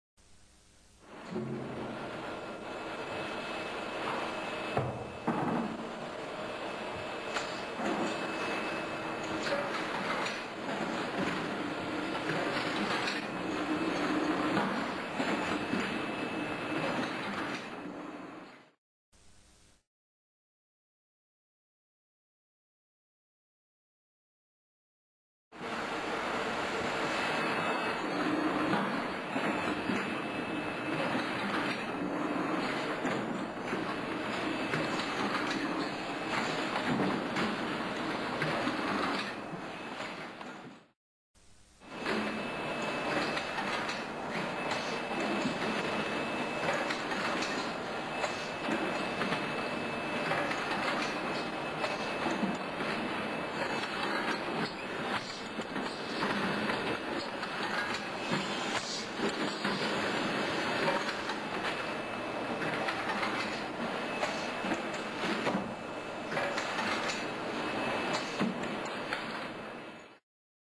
AG-PAK AP-60 - Scale Weigher - Hopper.wmv
This is a video from the early 1990s showing the operation of our infeed elevator, scale weigher and AP-60 Bagger.